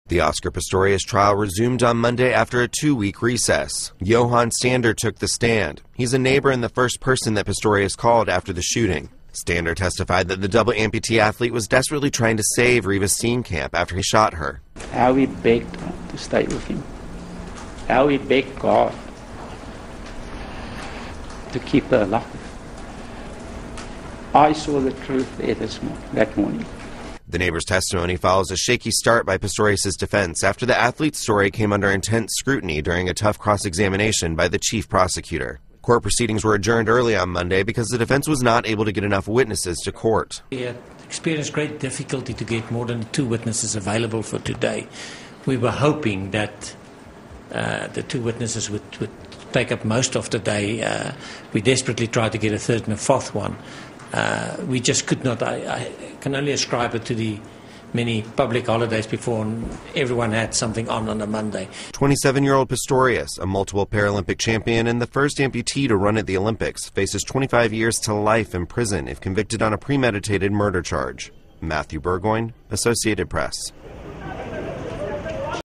Pistorius Trial Resumes After Two-week Recess
The Oscar Pistorius murder trial resumed after a two-week recess with the defense team calling Pistorius' neighbor to the stand, in an effort to bolster the athlete's case that he killed his girlfriend by mistake last year. (May 5)